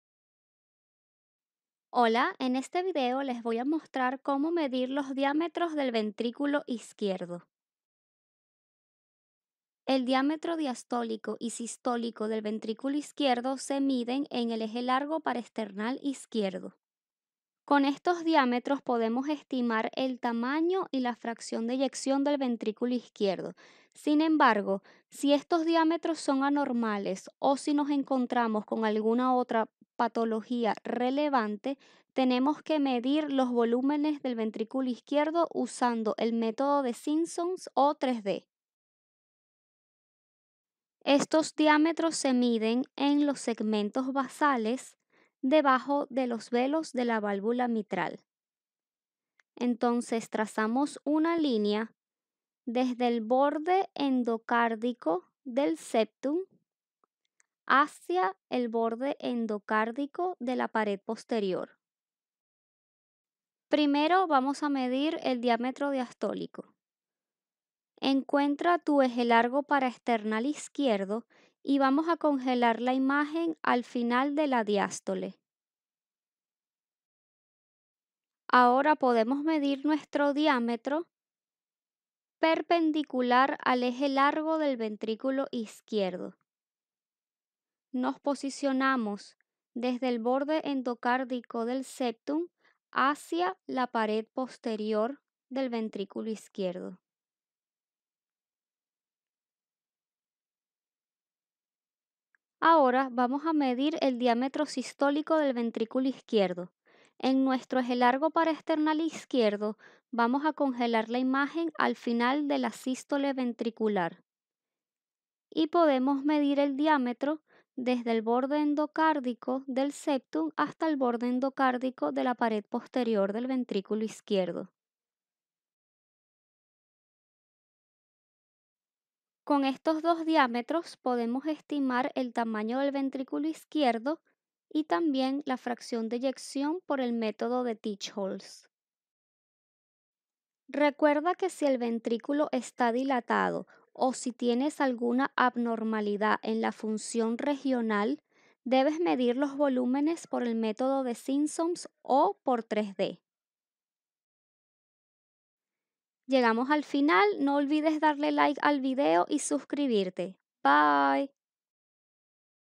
Como medir los DIAMETROS DEL VENTRICULO IZQUIERDO: Ecocardiografia!
Hola chicos, en este video les muestro como medir los diametros del ventriculo izquierdo (diastolico y sistolico). Estas medidas se utilizan para estimar el tamano del ventriculo izquierdo y la fraccion de eyeccion por el metodo de Teich-Holz.

Recuerden que en la presencia de cualquier anormalidad, debemos incluir mas mediciones como los volumenes.